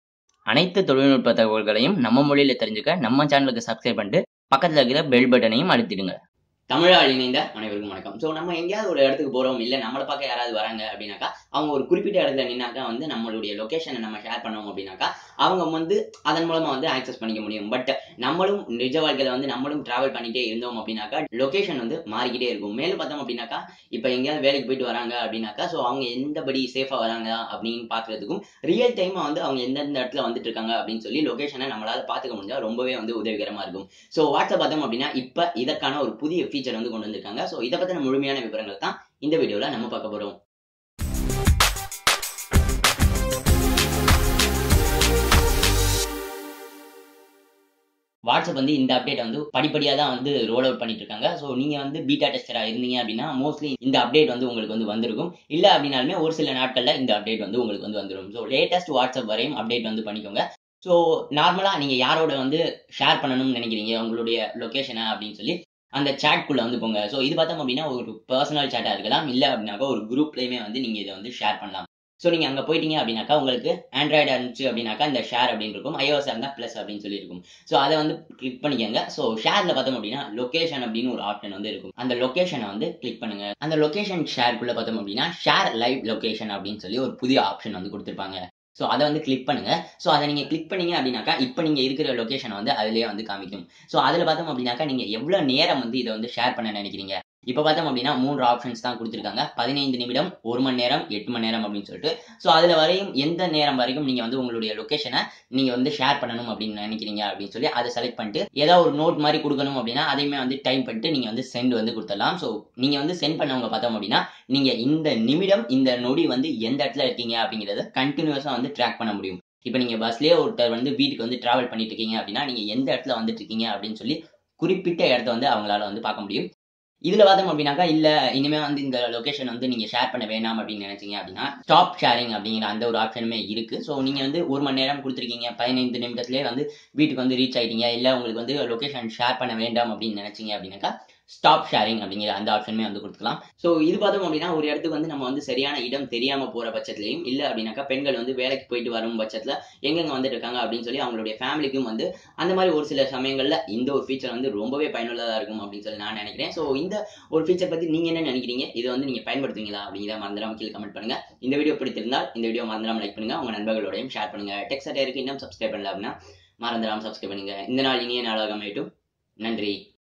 WhatsApp-ல் உங்களுக்கு தெரிந்தவரின் Locationஐ LIVE TRACKING செய்வது எப்படி? | Tech Satire
WhatsApp Live Location Sharing - Track Anyone's Location just with their WhatsApp Number | Tips & Tricks in Tamil | Tech Satire